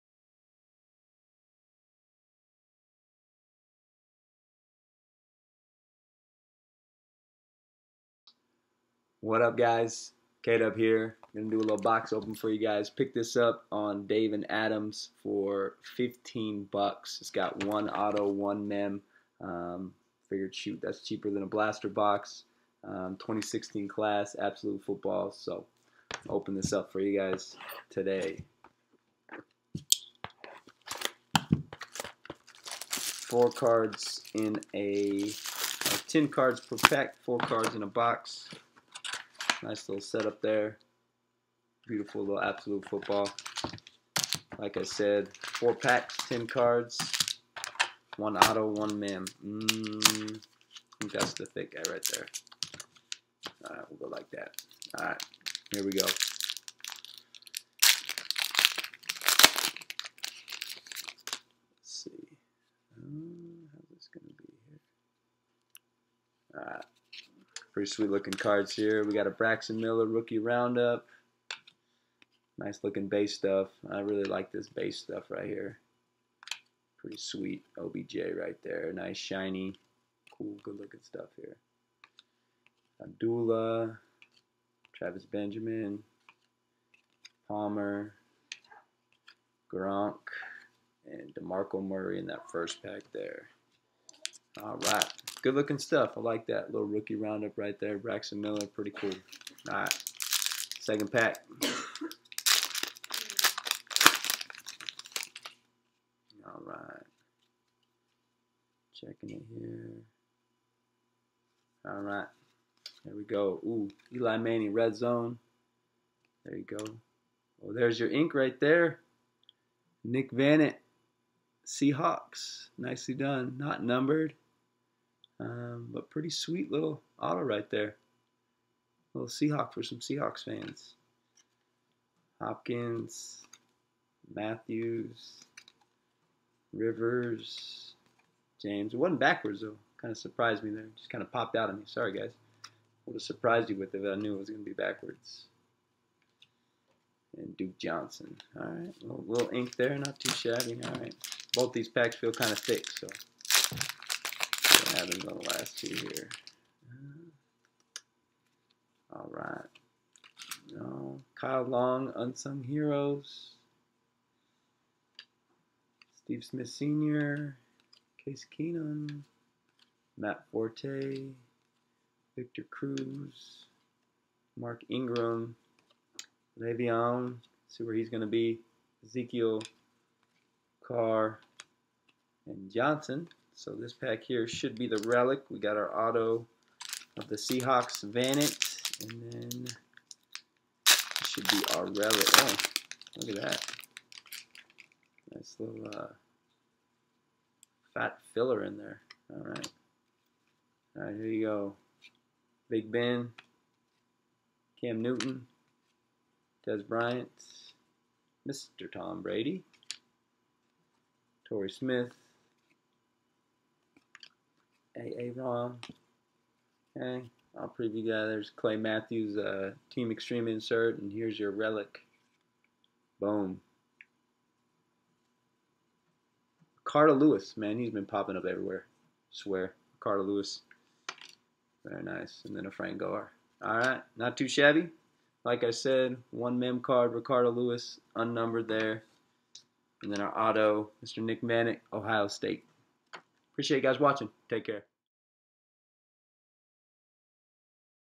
2016 Absolute Football Mega Box Opening
2016 Absolute Football Mega Box
1 autograph & 1 memorabilia car per box
10 cards per pack / 4 packs per box
Hit me up on Twitter: @MrKdub